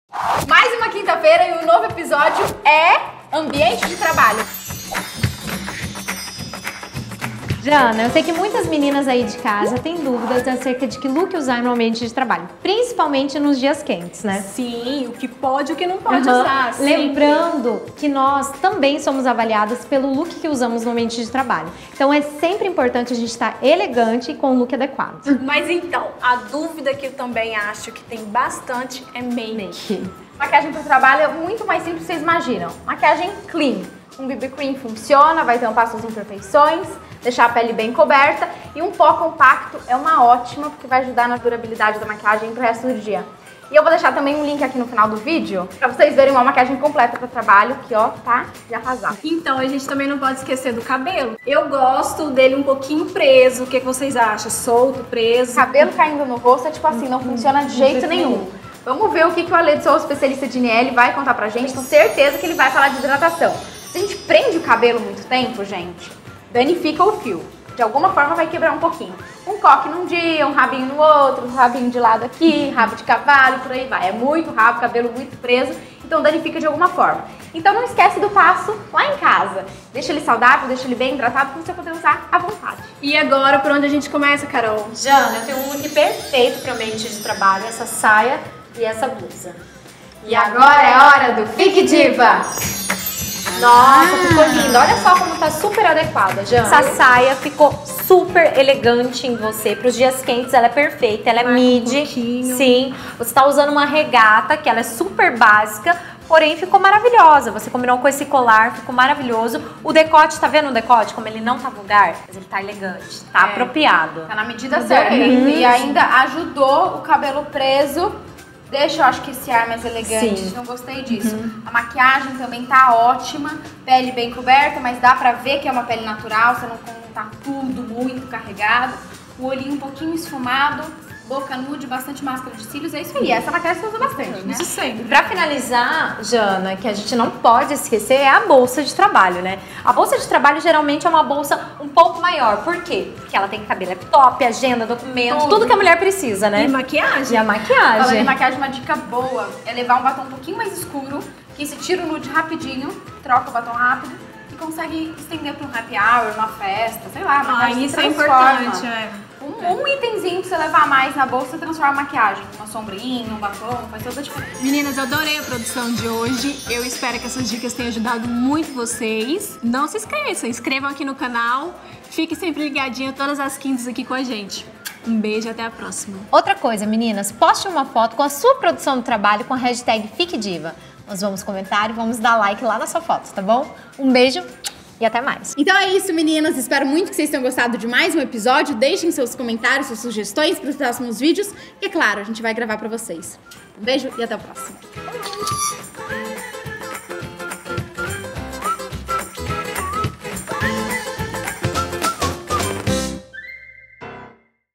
LOOKS PARA O AMBIENTE DE TRABALHO EM DIAS QUENTES COM MARIANA SAAD- FIQUE DIVA #6 // FHITS TV
LOOKS PARA O AMBIENTE DE TRABALHO EM DIAS QUENTES

CADASTRE-SE NO NOSSO CANAL!

Baixe o aplicativo do F*Hits na Apple Store e no Google Play no Android.

Nesse episódio, Carol Tognon e Mariana Saad deixam a Janaína Carvalho diva! O tema de hoje é ambiente de trabalho em dias quentes. O que usar? Pode decote? Qual o tamanho da saia? E a maquiagem ideal? Dê play e fique ligado nas dicas para ficar diva!